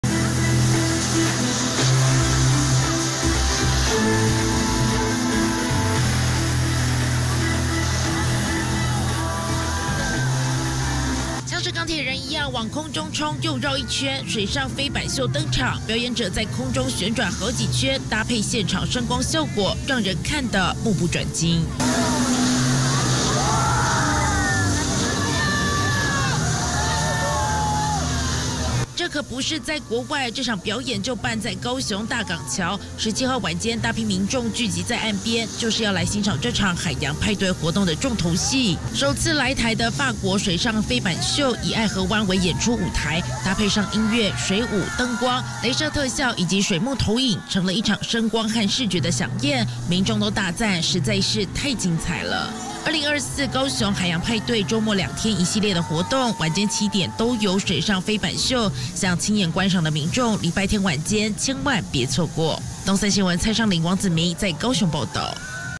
水上鋼鐵人！法國飛板秀首來台 炫技觀眾嗨翻@newsebc
法國水上飛板秀，在剛落幕的巴黎奧運中演出，緊接著來到台灣，在高雄的愛河灣進行表演，飛板秀成了海洋派對的重頭戲，大批民眾湧入，就是為了一睹這場有國際級水準的秀，搭配聲光效果以及水幕投射，帶給觀眾一場絕佳的饗宴。